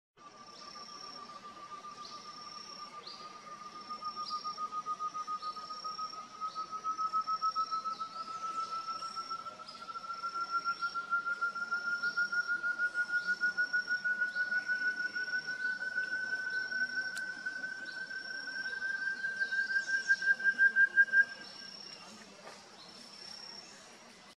Voice of Chamaeza meruloides (Tovaca-cantadora, Such’s Antthrush) Brazil by Antonio Silveira.
Recorded in Cantareira State Park, S.Paulo state, Brazil,12-2023 by Antonio Silveira.